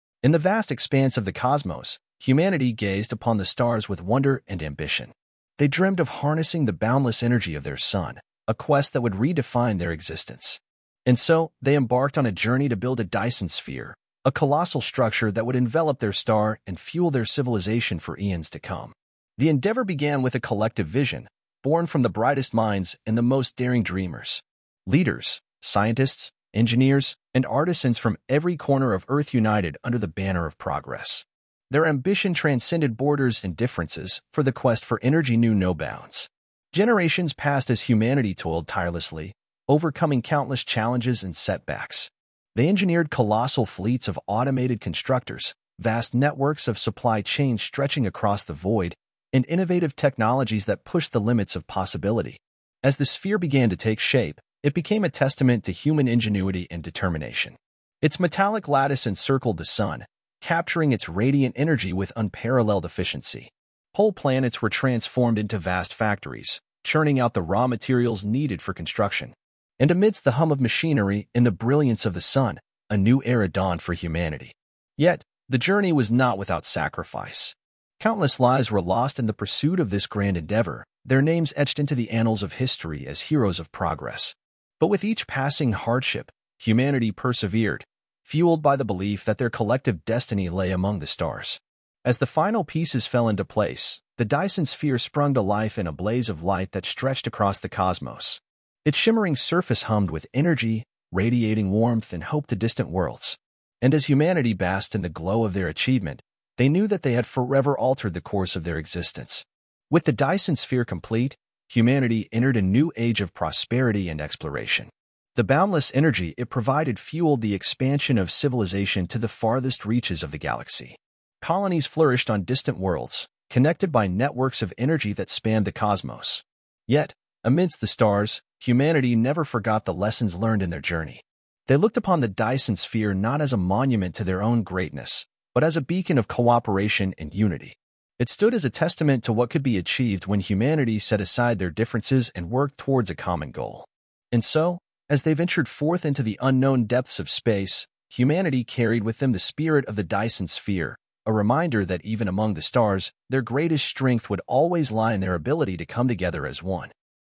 Building a Dyson Sphere: Humanity's Greatest Engineering Feat
Join us on a journey through the cosmos as we explore the incredible undertaking of constructing a Dyson Sphere around our sun. From the initial conception to the final assembly, witness the triumphs, challenges, and ingenuity of humanity as we harness the power of our star like never before. Discover the science, engineering marvels, and the unity that propelled us towards this monumental achievement. The Dyson Sphere stands as a testament to human ambition and cooperation, forever altering the course of our civilization among the stars.